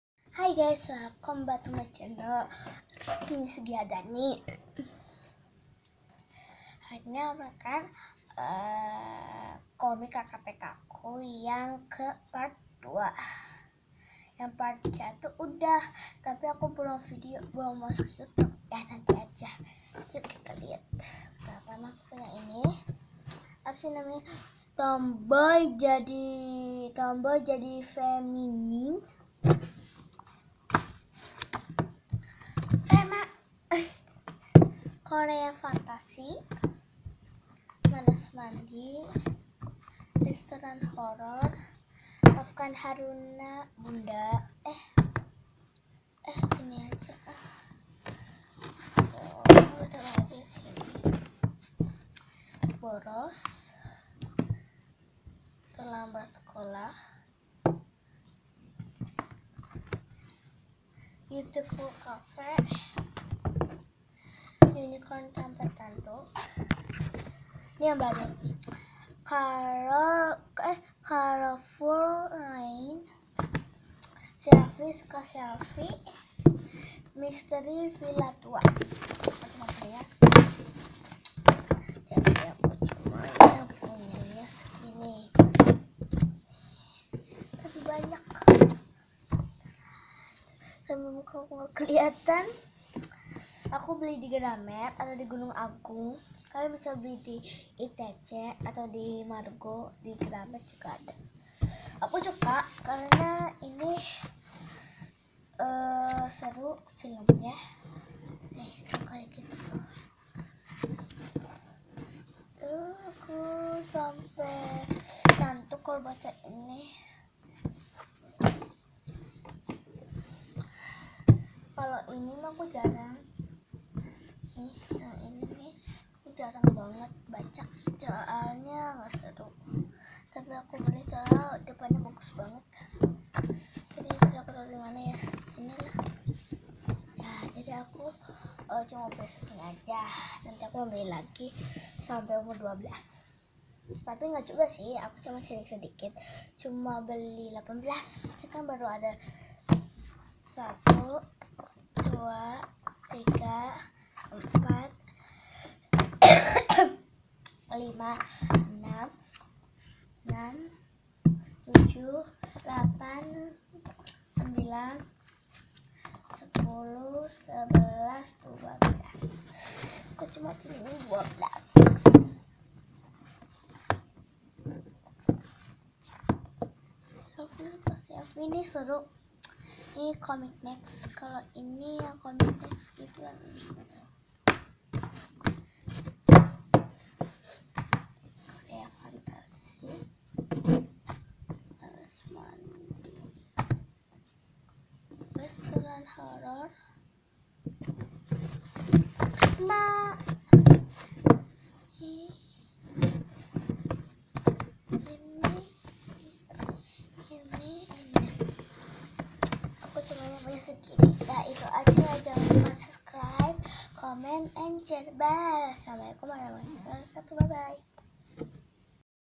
Komik kkpk ,part 2
Komik aku ada 12  ,  dan aku seneng dibeliin komik kkpkp selesai